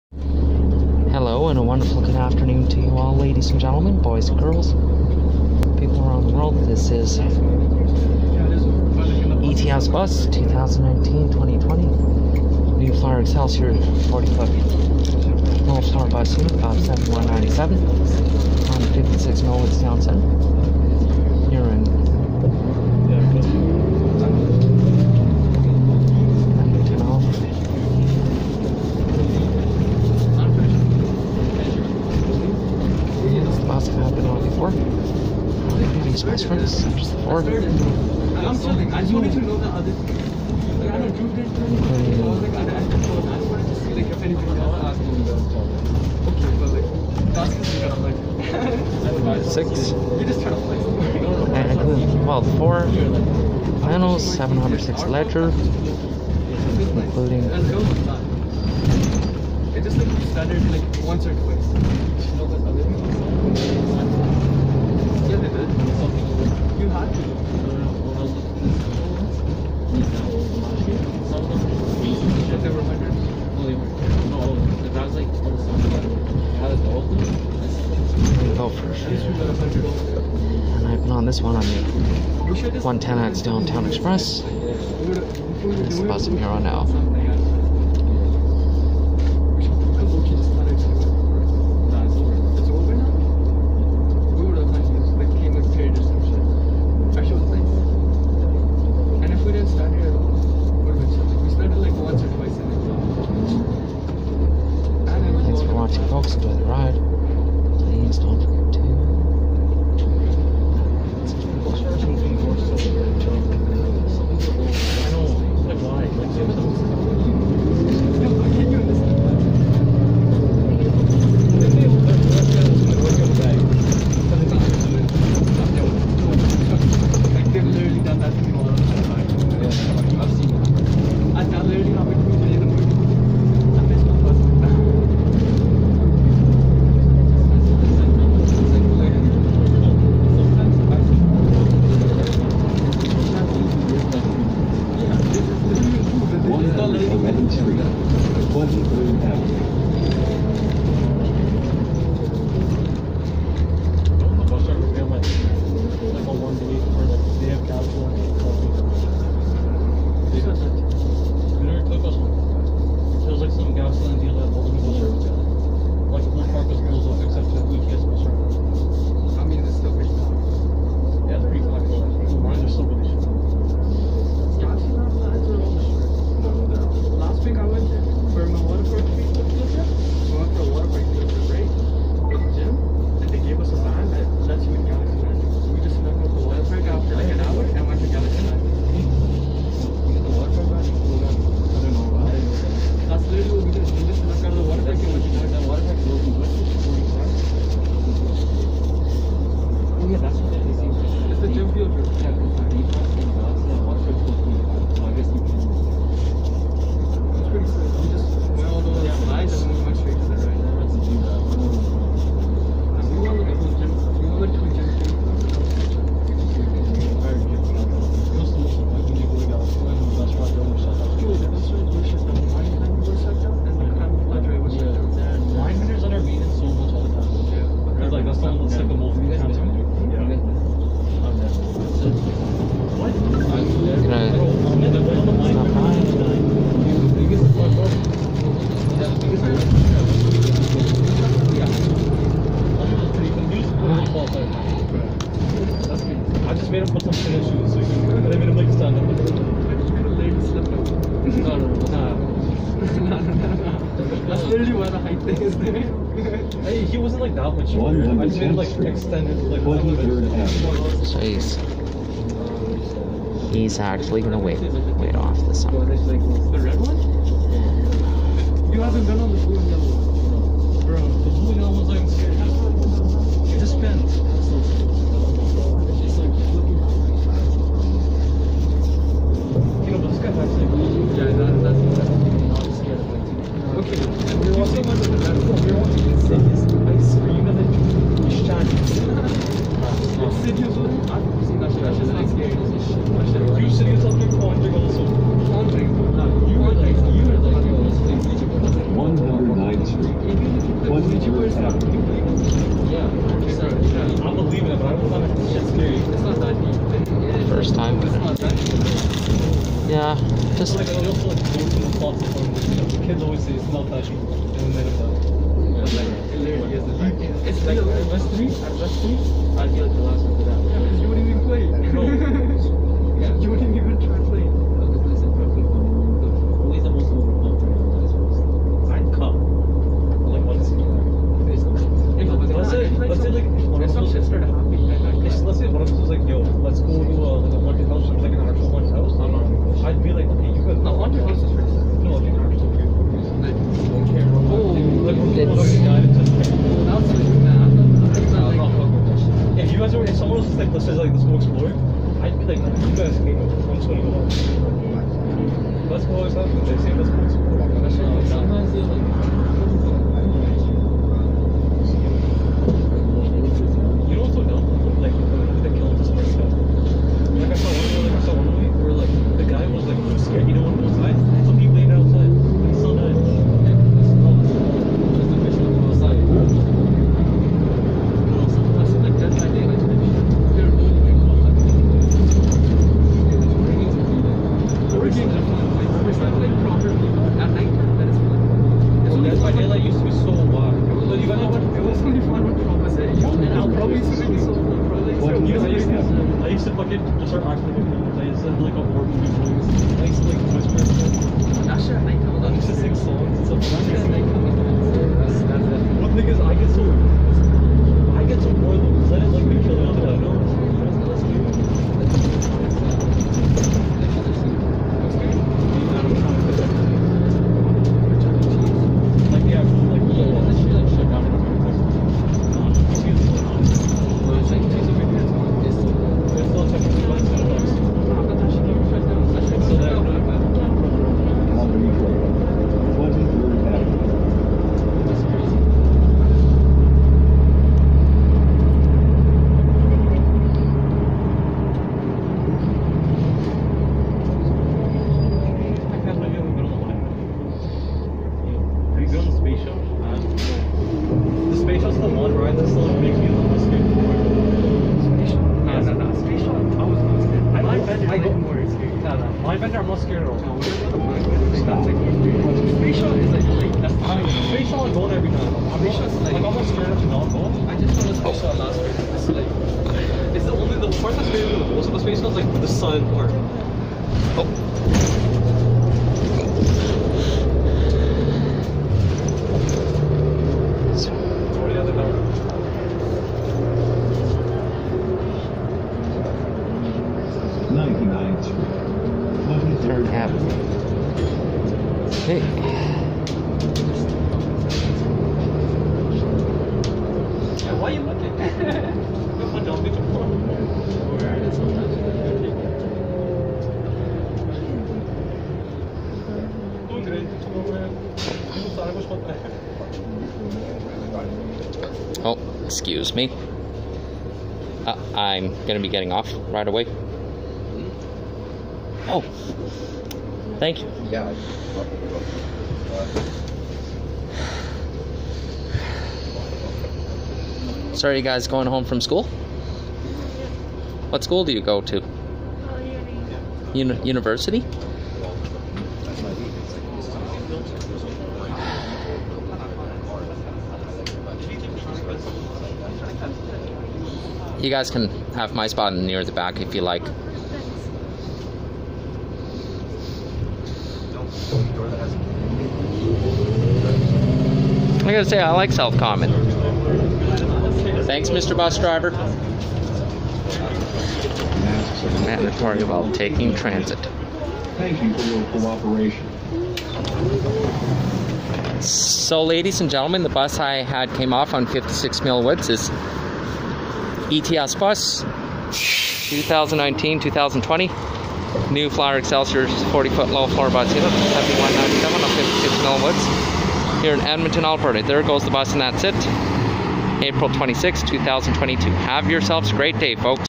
ETS Bus 2019/2020 New Flyer XD40 #7197 On 56 Millwoods
From Century Park Transit Centre To 99th Street & 23rd Avenue, South Common, in Edmonton Alberta.